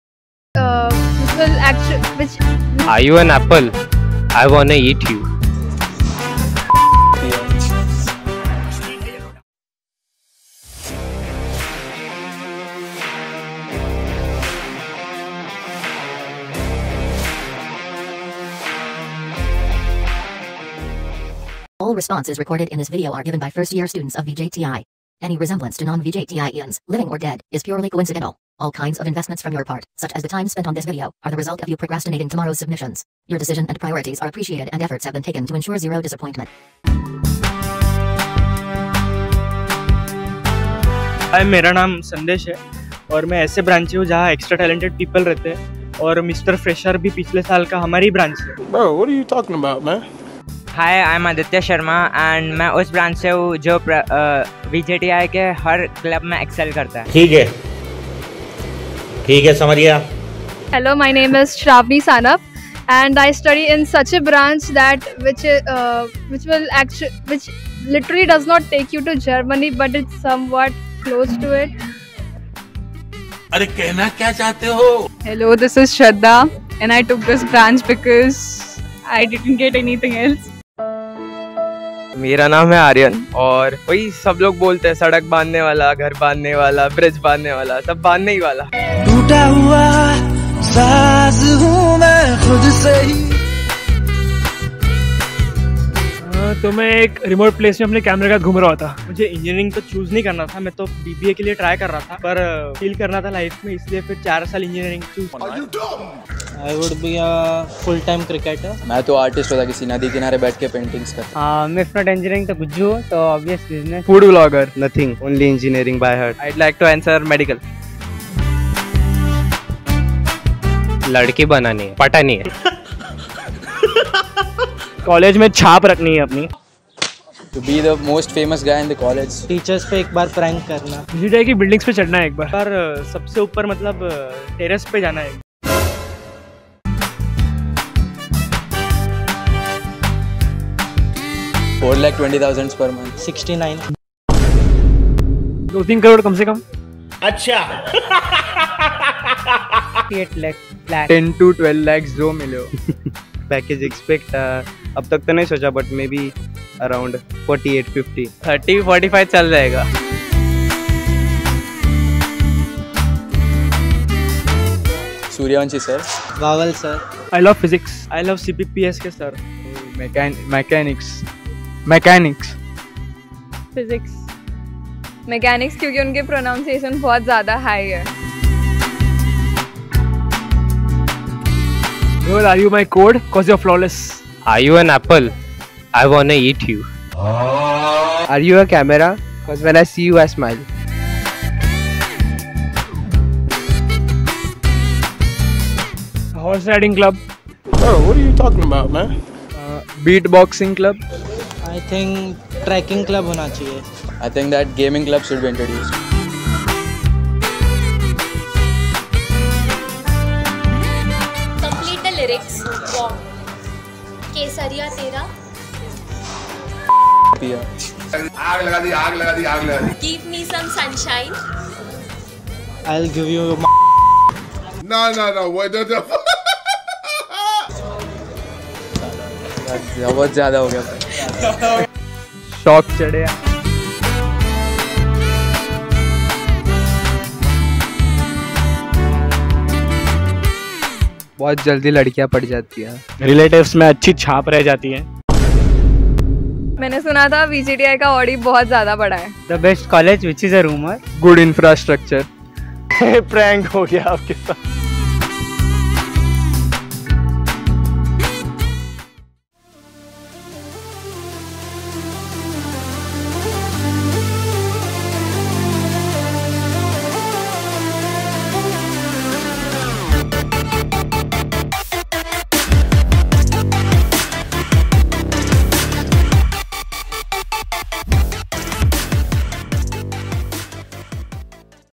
Freshers' Introduction 2022 | DLA VJTI
Youth,
What is it,
A bit timid,
A bit brave,
Alot cool,
And so much beautiful.🌸

Introducing to you our chucklesome, chic and youthful First Years🔥

With this illustrious zeal, and so much to say, only a little can be portrayed in a teaser.🥺 So show some love and tighten your seatbelts, because we are about to dive🎢 right into the DLA FRESHERs INTRODUCTION.✨

Credits:
Content Credits- DLA Team
Vedangi Patil
Siddhant Chandan
Srushti Pol
Ira Sheth
Chinmay Mundane
Amey

Videography & Video Editing- Digital VJTI Team
Atharva
Aashish
Yashraj
Omkumar
Abhishekh
Rishi
Paras
Samidha